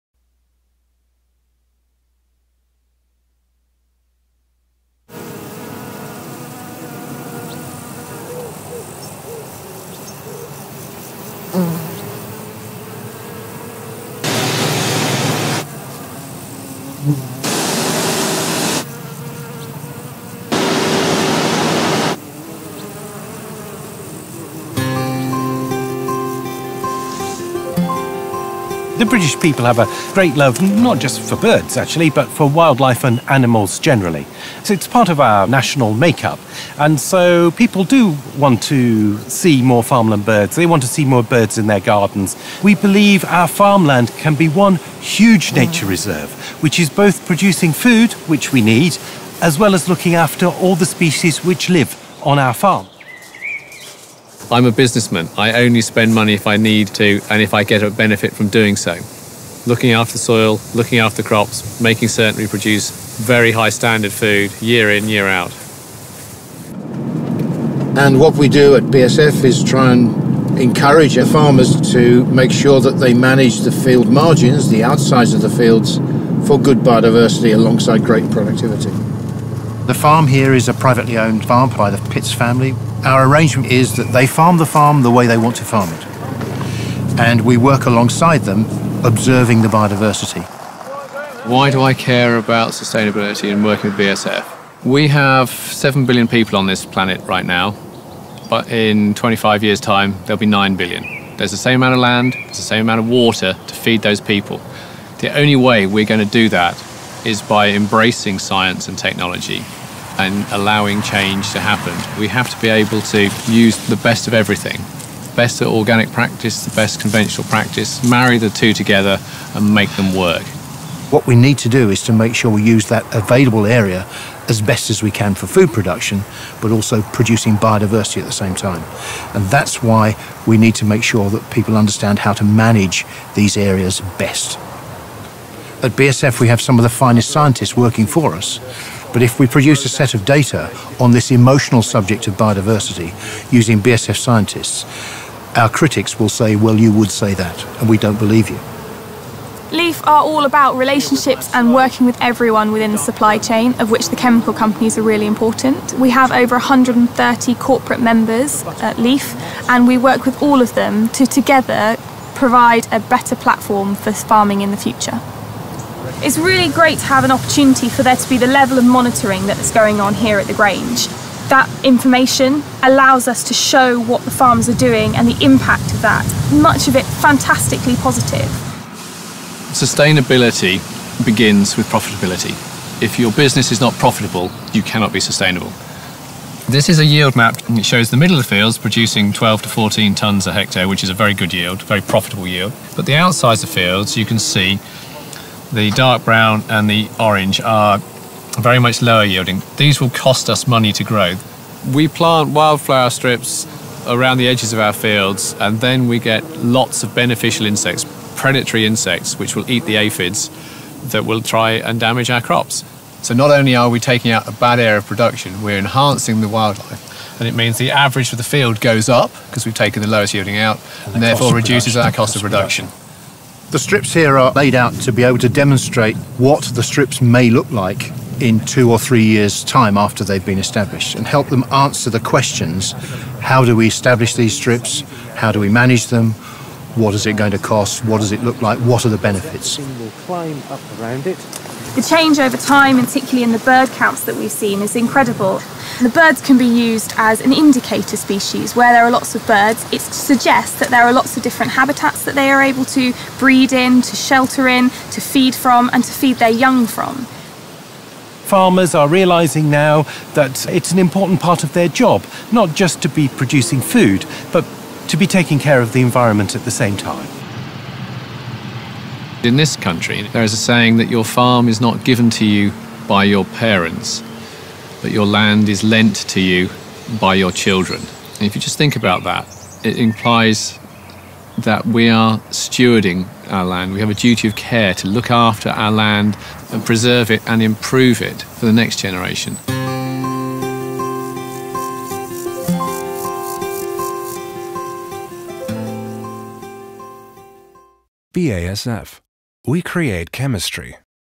GSF Awards 2015 | BASF | Biodiversity - making it work on a modern farm
How can nine billion people be fed while nature is protected? Conventional agriculture and biodiversity go here more and more hand in hand. BASF initiated the Farm Network, a partnership with farmers, experts for agriculture and for nature conservation to master this challenge. „Sustainability begins with profitability“, declares Andrew Pits, English farmer in the third generation and member of the Farm Network. Together they seek to achieve a sustainable balance between the economic demands of modern farming and the protection of biodiversity. They precede as a role model trying to motivate other farms to join in!

The report is based on quotes and filmed in a documentary style without any comment text. People affected report from their own perspective and in their own words about difficulties and success they had in finding a way to be sustainable and profitable at once.

Trailer, Report and Infographic show how modern agriculture and sustainability can find their way together. BASF publishes on internal and external communication channels based on an integrated distribution concept and in connection with international events with relevant topics, e.g. the “International Day of Biodiversity” on May, 22nd. The focus lies on online media and tv.’